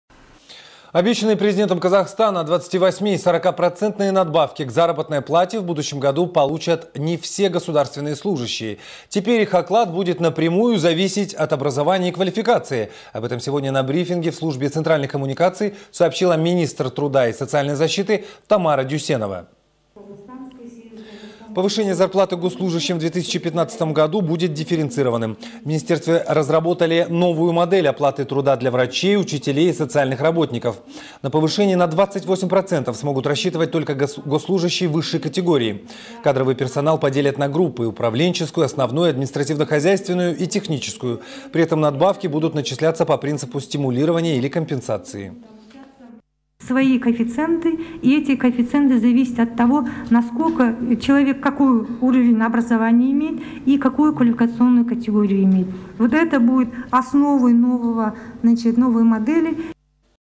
Оклад госслужащих будет зависеть от образования и квалификации
Обещанные Президентом Казахстана 28- и 40-процентные надбавки к заработной плате в будущем году получат не все государственные служащие. Теперь их оклад  будет напрямую зависеть от образования и квалификации. Об этом сегодня на брифинге в Службе центральных коммуникаций сообщила министр труда и соцзащиты Тамара Дуйсенова.